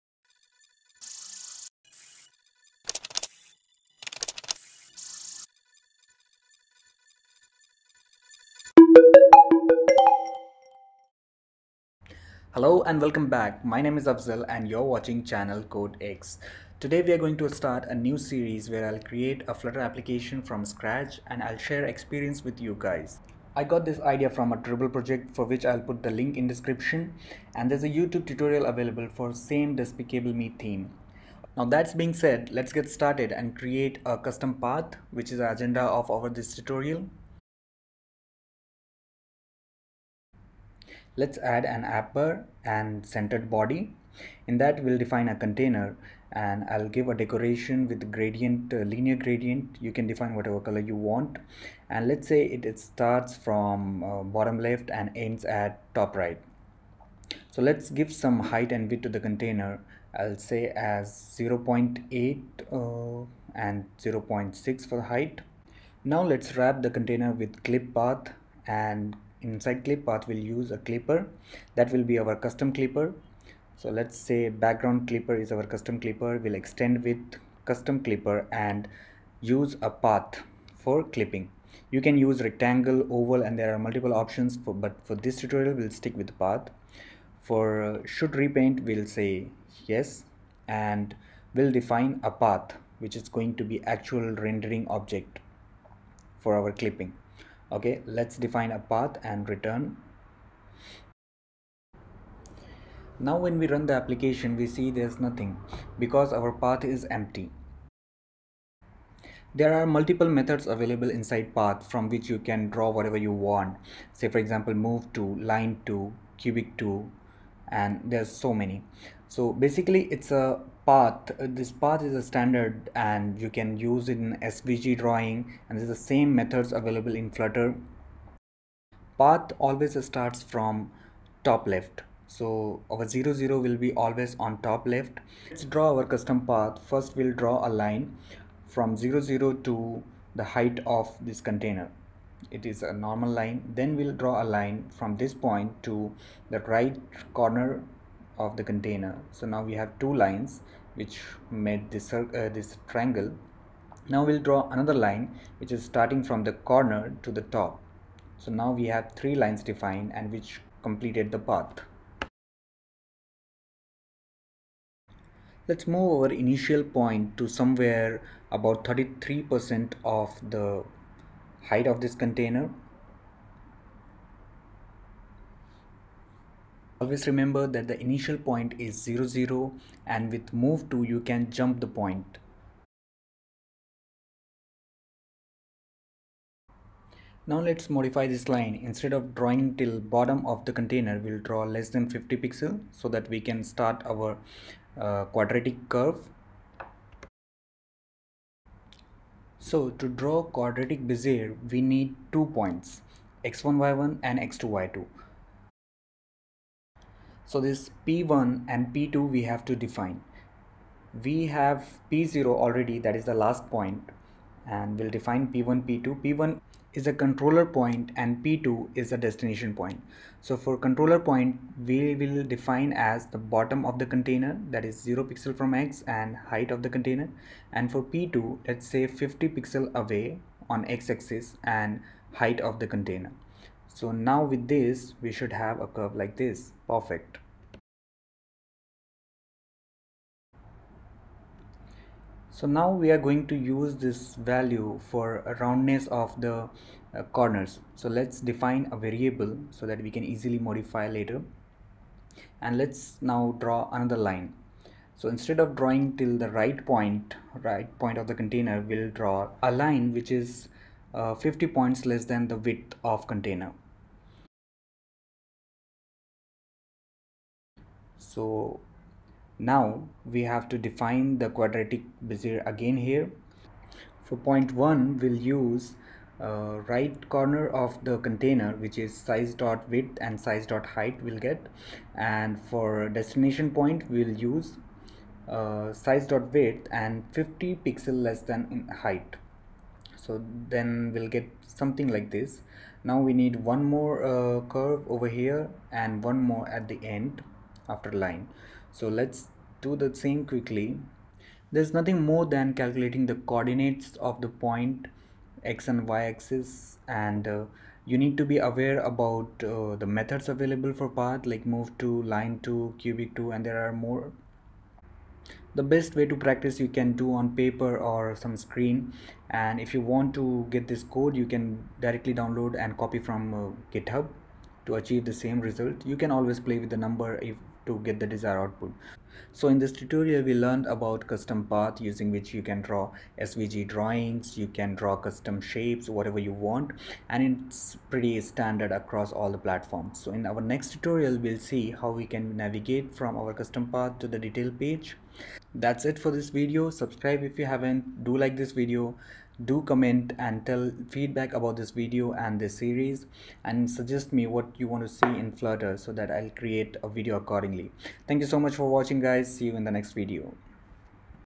Flutter Custom Path Tutorial || Part 1 (A Path for Clip)
Welcome to yet another series of Flutter.

In this series we will create flutter application from scratch and complete the app by end of the series.

MCU App | A path for Clip

In this tutorial we will see basics about Path and custom path clipping in flutter.